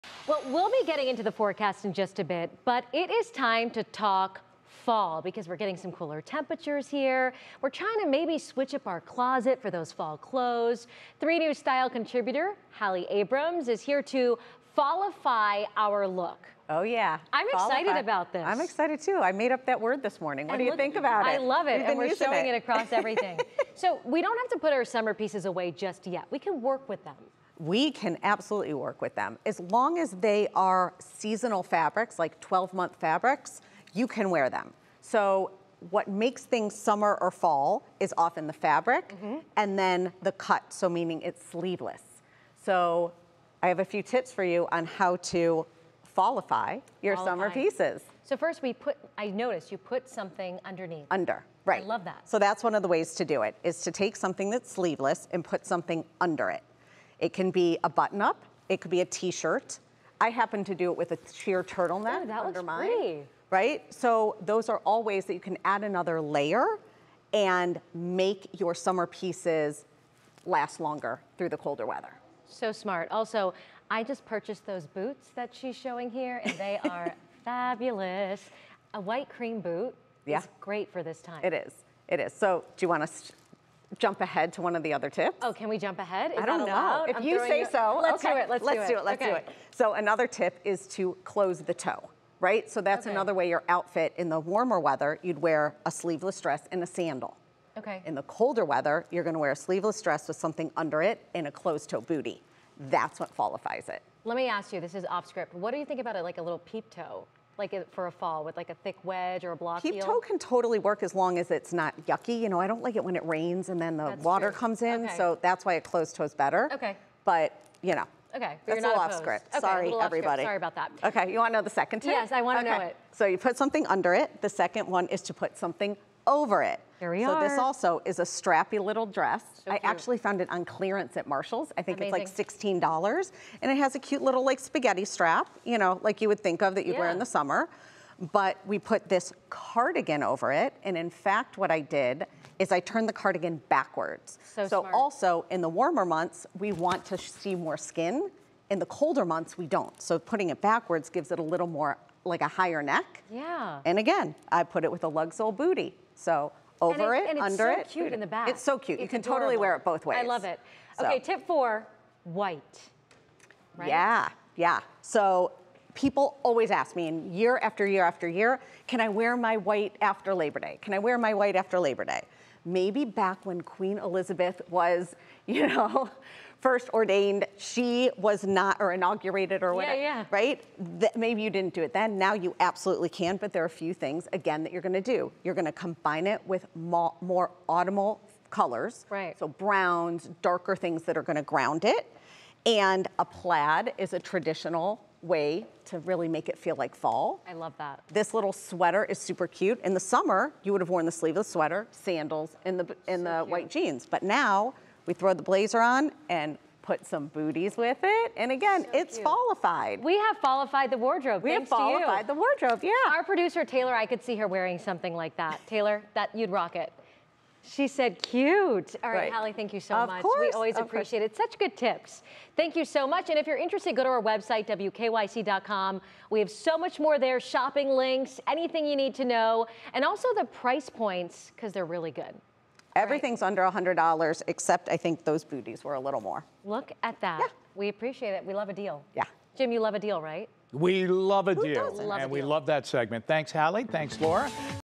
How to turn your summer styles into fall fashion favorites
The changing seasons can make for a challenging fashion transition. But you don't have to put all of your summer styles away to make room for fall. 3News Style Contributor Hallie Abrams stopped by the "Front Row" set to share some easy tips on how to "fallify" your favorite looks.

Tip 1: Style a shirt "under it"

Shop the look:

Calvin Klein Dress $39.99 at Marshall’s
Slim Cut White Button Down Shirt $89.50 at J. Crew
Steven Sock Bootie - $49.99 at Marshall’s
Tip 2: Add a jacket or sweater "over it"

Shop the look:

Sienna Sky Dress on clearance $16 at Marshall’s
Moon and Madison Sweater $16.99 at Marshall’s
Chelsea Lug Sole Boot $228 at Madewell
Tip 3: Say goodbye to sandals and "close the toes"

Shop the look:

Michael Kors Suede Chelsea Boot $70 at Marshall’s
Tip 4: You can wear white after labor day by pairing autumnal colors and textures

Shop the look:

Ecru Classic Straight Jean $99.99 at J. Crew (comes in petite and tall)
Brown Button Front Sleeveless Sweater $68.00 at Madewell
Plaid Double Breasted Blazer $178 at Madewell

Read more about turning those summer styles into fall fashion